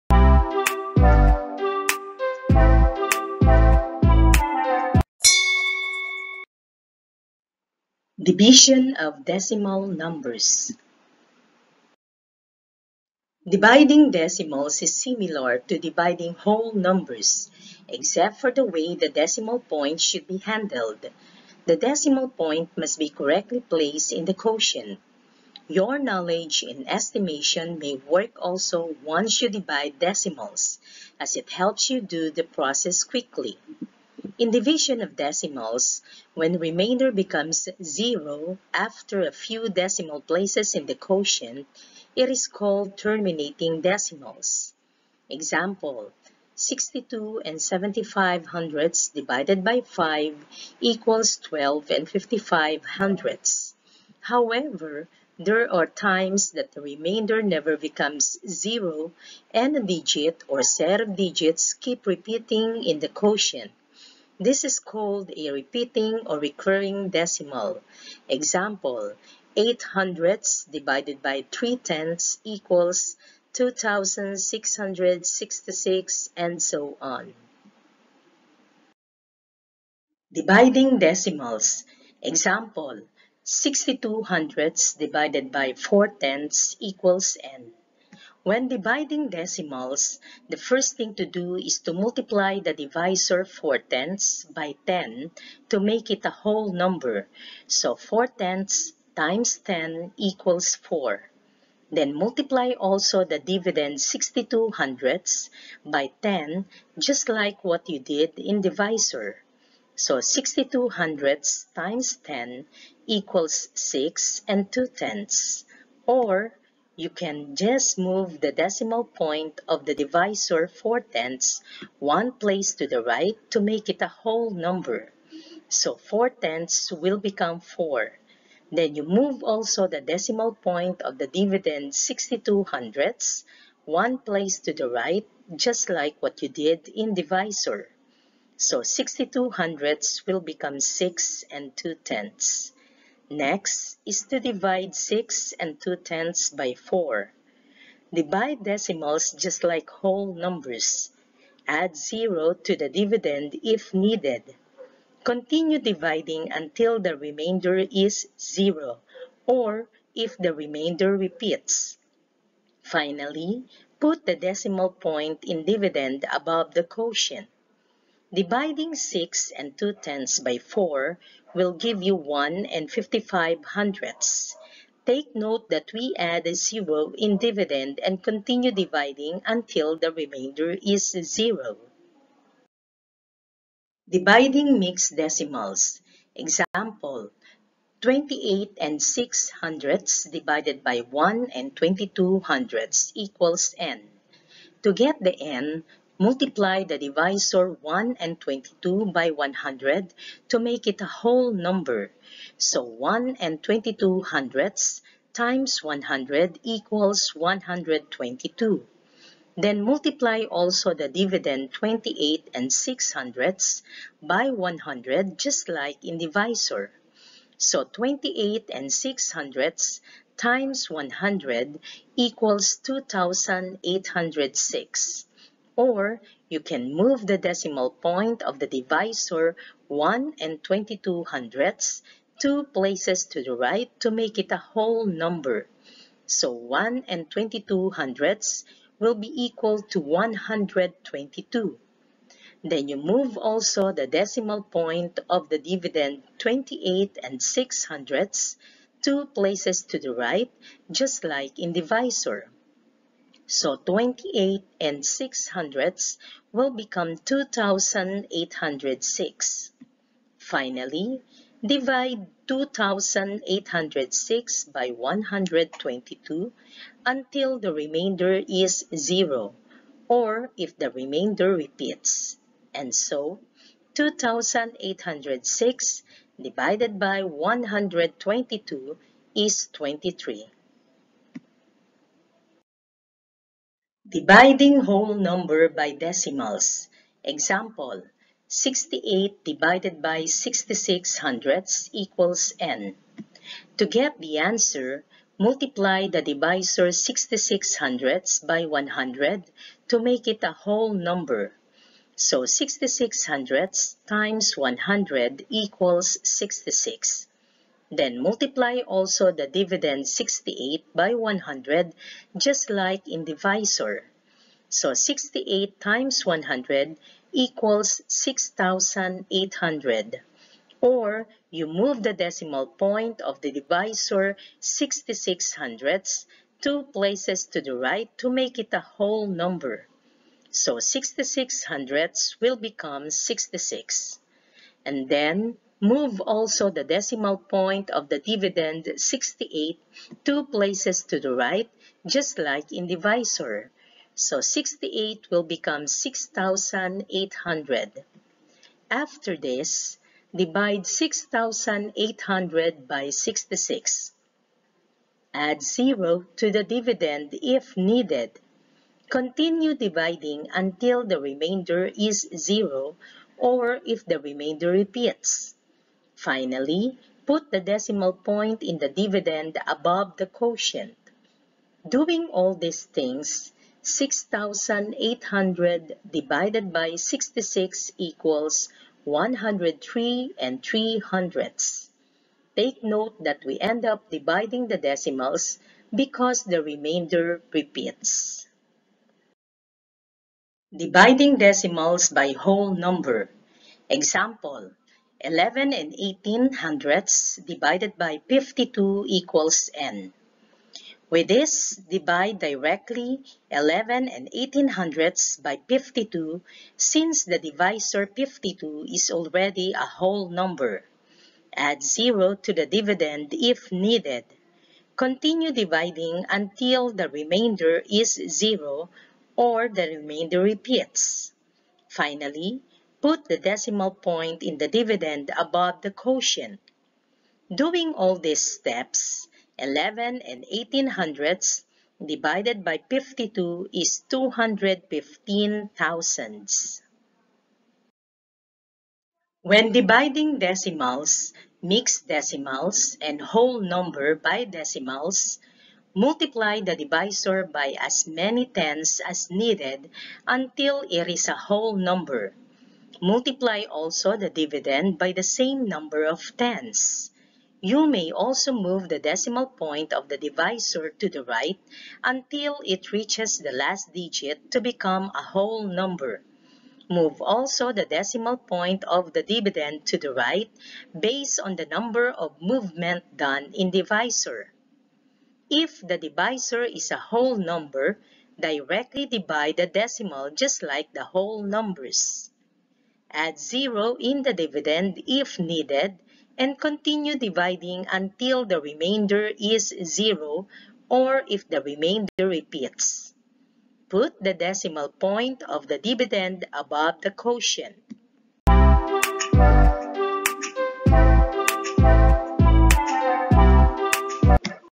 Division of Decimal Numbers
In this lesson you will learn how to divide decimals, mixed decimals, and whole number by decimals and vice versa. The strategies and steps indicated will help you do it.